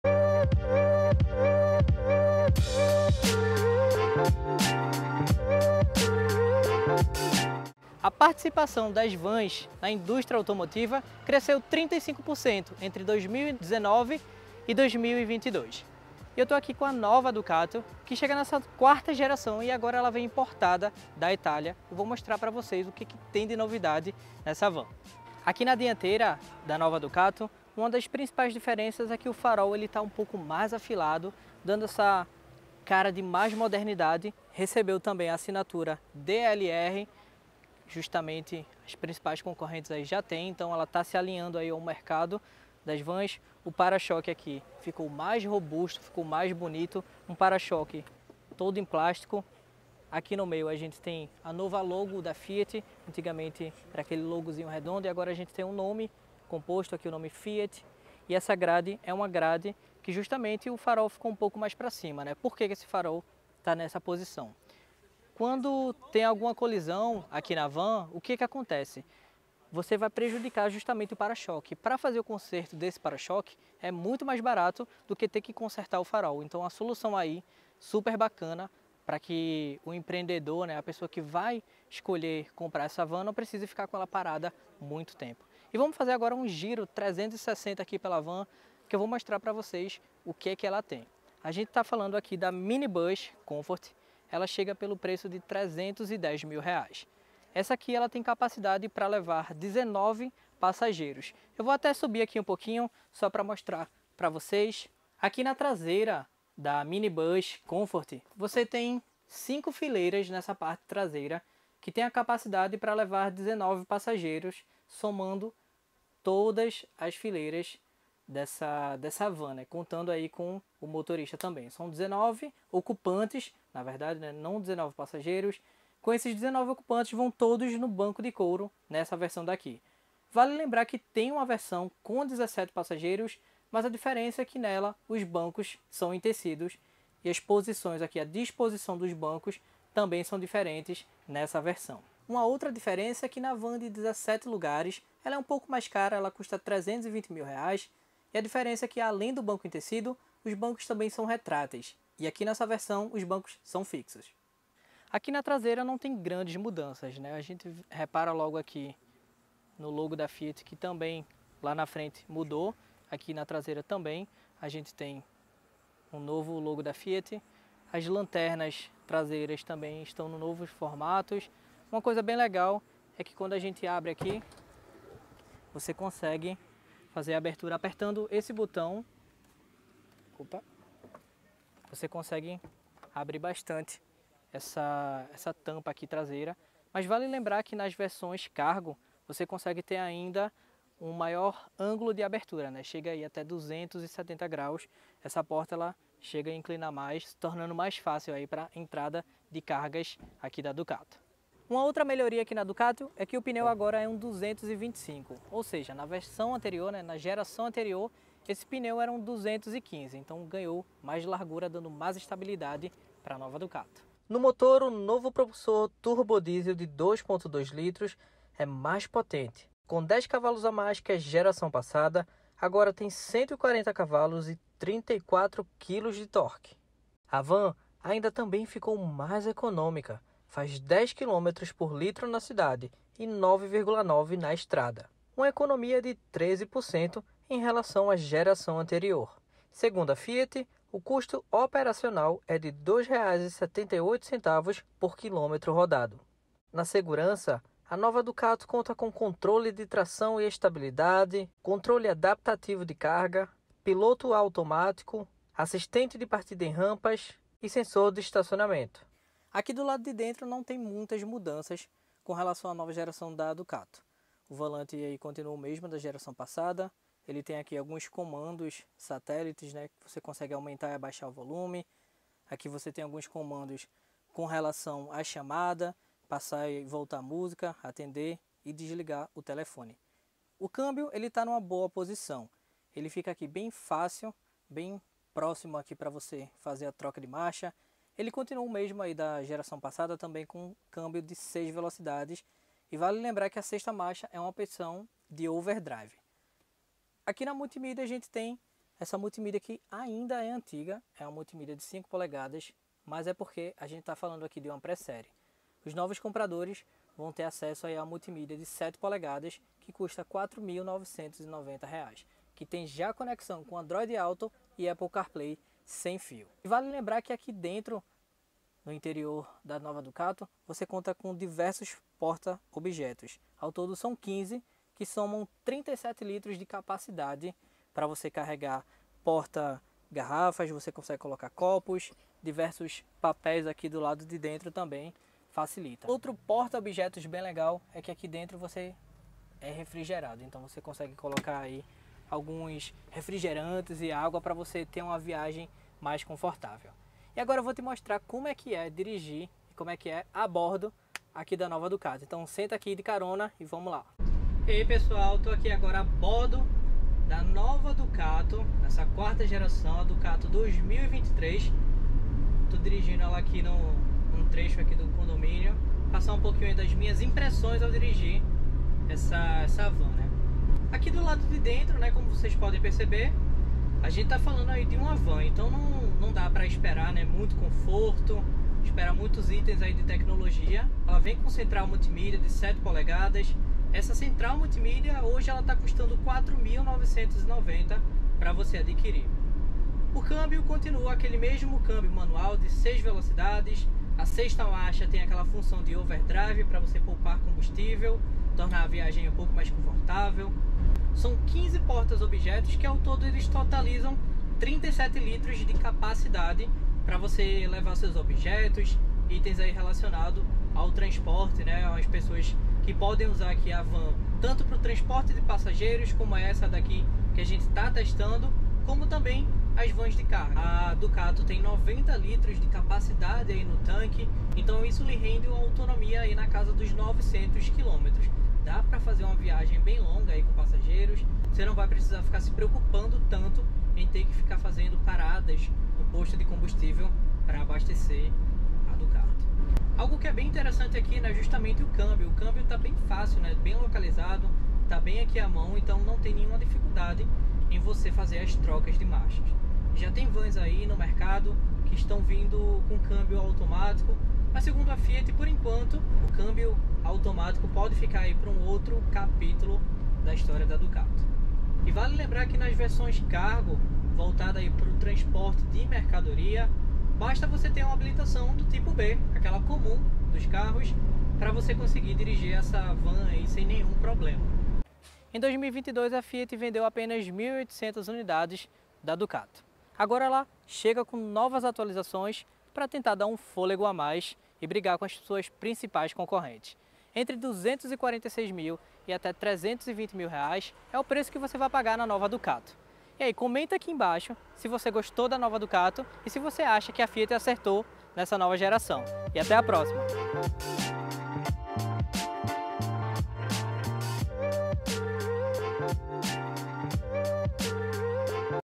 Nova Fiat Ducato quer ser a melhor van do mercado. Veja o que ela oferece a partir R$ 245.990
A nova Ducato chega na sua quarta geração com atualizações importantes. O motor agora tem 140 cv de potência e, ainda assim, ficou 13% mais econômico. São cinco versões disponíveis entre opções para transportes de cargas e também para passageiros.

No visual, a van ganhou DRL e uma grade frontal mais robusta. Além disso, conta com alguns itens de segurança, como o assistente em rampas, piloto automático, limitador de velocidade, controle de estabilidade (ESP), controle de tração (TC) e o LAC, um controle adaptativo de carga que mede o centro de gravidade para garantir a estabilidade lateral do veículo.

O Novo Fiat Ducato estará disponível para venda na rede da Fiat a partir do dia 5 de abril. Confira abaixo os preços de cada versão:

Novo Fiat Ducato Cargo: R$ 245.990
Novo Fiat Ducato MaxiCargo: R$ 249.990
Novo Fiat Ducato Multi: R$ 261.490
Novo Fiat Ducato Minibus Confort: R$ 309.990
Novo Fiat Ducato Minibus Executivo: R$ 319.990

NOSSO CANAL: Youtube.com/jorgemoraes